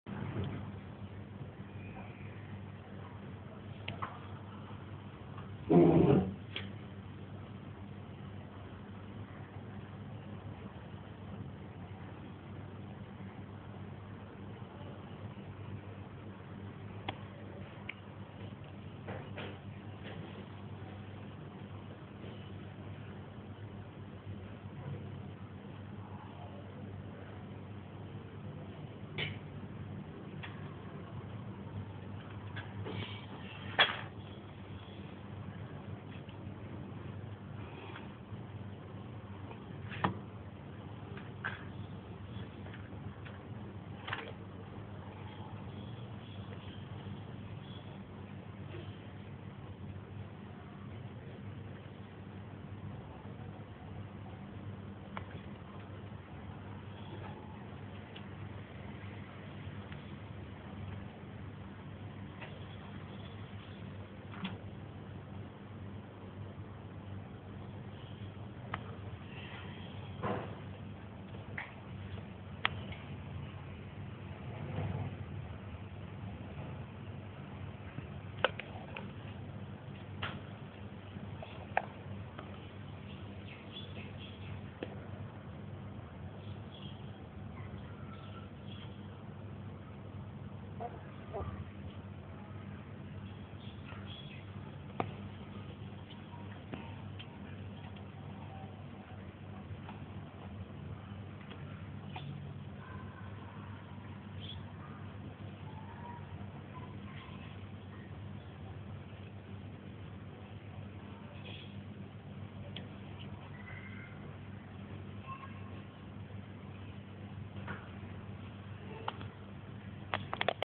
Bunny lam, exploring his new toilet
He is just curious about his new toilet and peed on it after a while to show that he knows its a toilet